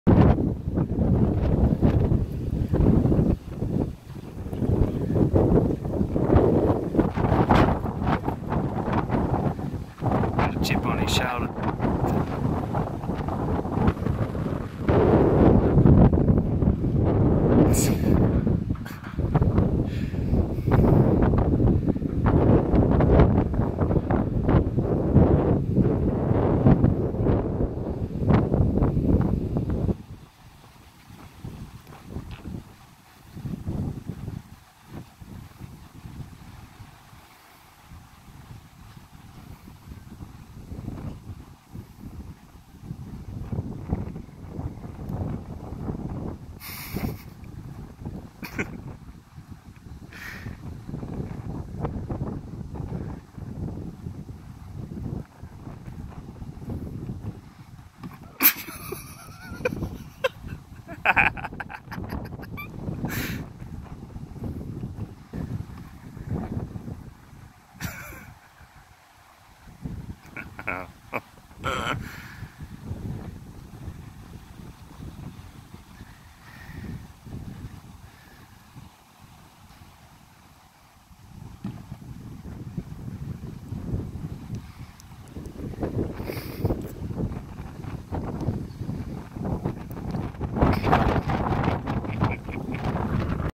Ram Charges at Bucket in Baa-gressive New South Wales Stand-Off
Credit: Jobe Goman via Storyful